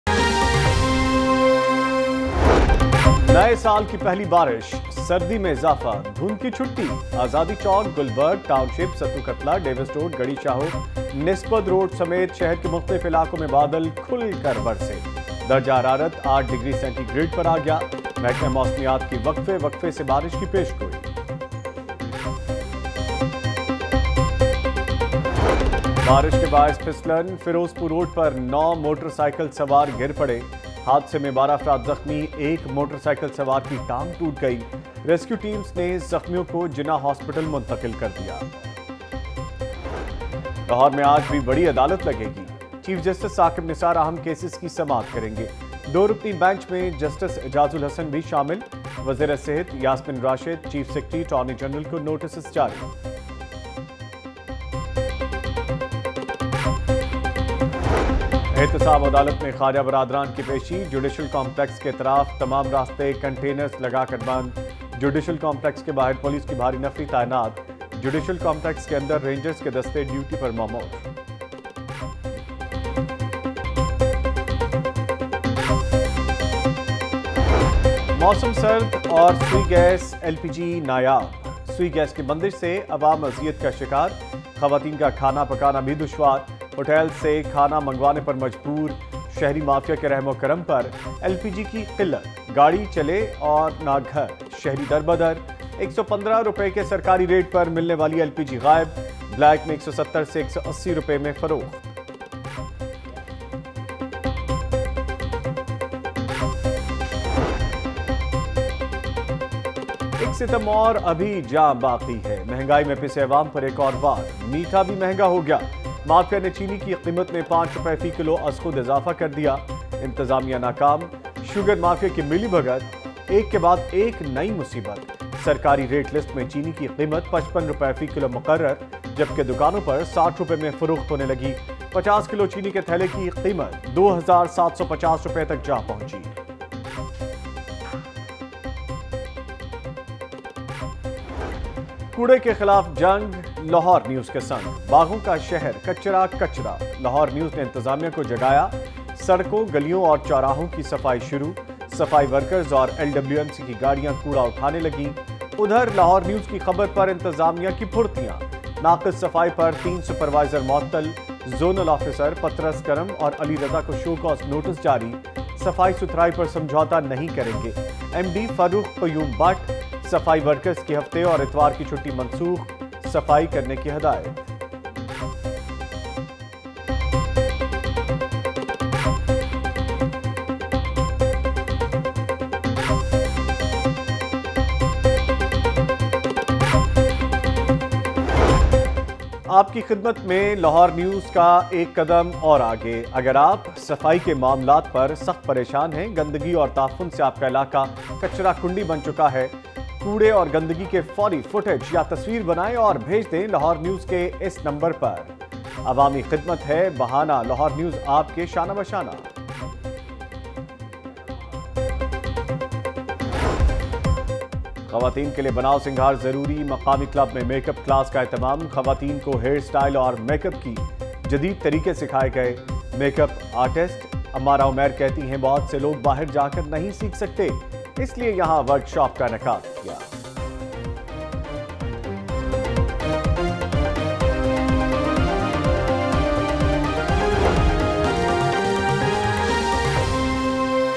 09 AM Headlines Lahore News HD – 5th January 2019
Watch Latest News:
Maulana Tariq Jameel suffers heart attack, 8-year old dies after falling from roof،Petrol prices decrease by four rupees, Naan price increased by two rupees, LHC orders No petrol without helmet, Bollywood reaction on Maula Jatt trailer, Karan Johar applauds Fawad Khan’s performance in Maula Jatt, LHC orders E-challan on helmet violation, Trailer of Maula Jatt released, Minar e Pakistan closed for public,  Basant Festival to be celebrated in Lahore again, PTI Govt removes ban from Basant Festival, Khokhar Palace declared illegal, Hamza Shehbaz throws water bottle at Deputy Mayor, PTI Govt replaces vehicle registration book with smart card, Helmets made compulsory with motorcycle purchase, NAB approves 10-days remand of Khawaja Saad Rafique in Paragon Scandal , Sanitation tax to imposed on locals according to marla, Gunah Tax imposed on cigarette smoking, Crickter Yasir Shah breaks 82-year old record, Double helmet made compulsory on pillion riding, Lahore among worst polluted cities ,Gas shortage prevails in Lahore, , Petrol prices witness sharp increase, Poultry prices witness sharp increase, Dry Fruit prices increases, , Immense traffic jam in Lahore, Scuffle breaks out between locals and traffic wardens, Dollar prices witness sharp increase, Oil tanker collides at Waris Meer underpass and much more.